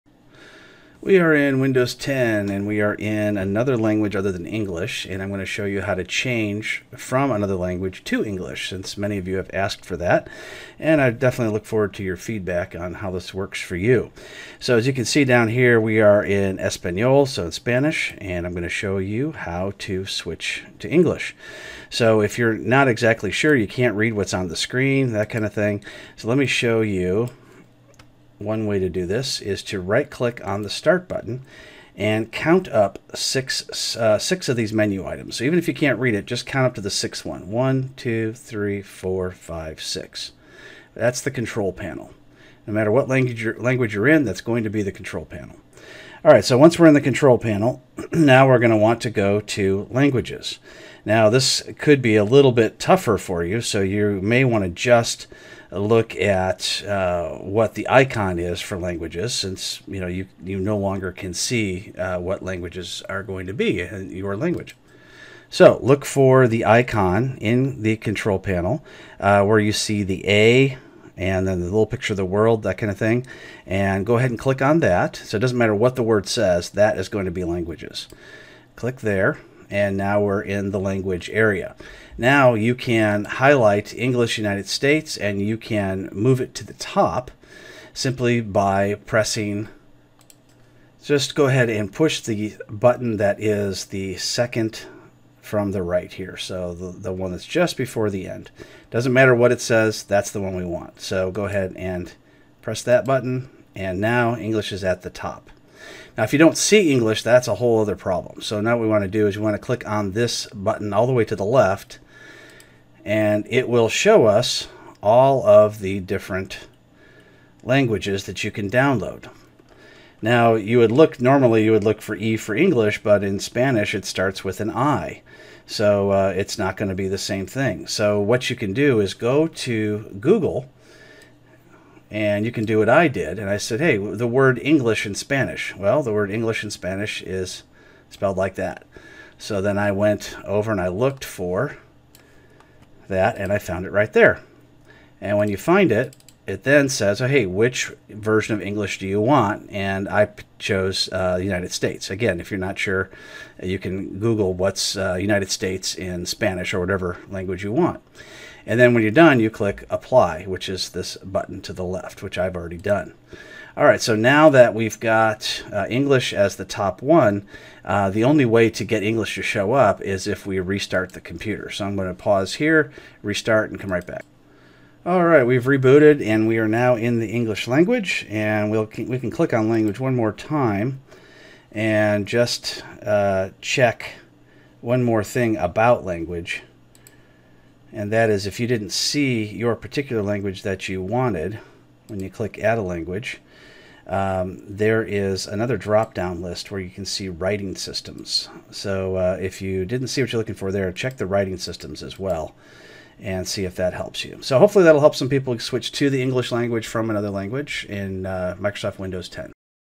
How to switch languages to English in Windows 10
Check out my book "This is IT" for true IT experiences during my long career

Author, teacher, and talk show host Robert McMillen shows you how to switch languages to English in Windows 10. This video shows how to change from Spanish to English, but the rules still apply when switching from any language to English.